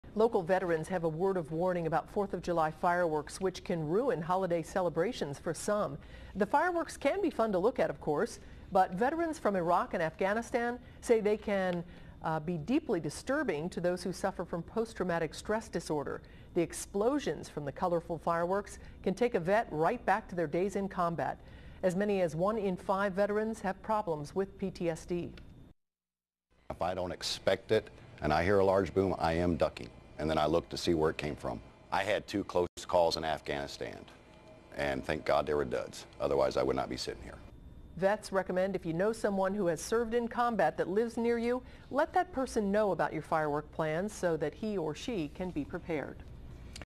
Fireworks can further traumatize some veterans
CINCINNATI (WKRC) -- Local veterans have a word of warning about Fourth of July fireworks which can ruin holiday celebrations for some. The fireworks can be fun to look at, but vets from Iraq and Afghanistan said they can be deeply disturbing to those who suffer from Post-Traumatic Stress Disorder.      The explosions from the colorful fireworks can take a vet right back to their days in the combat zone. As many as one in five veterans have problems with PTSD.   Mark Cornett, VFW post 657, said, If I don't expect it, and I hear a loud boom, I am ducking. And I look to see where it came from." He continued, "I had two close calls in Afghanistan, thank God they were duds. Otherwise I wouldn't be sitting here." Vets recommend if people know someone who has served in combat that lives near them, let that person know about the fireworks plans so he or she can be prepared.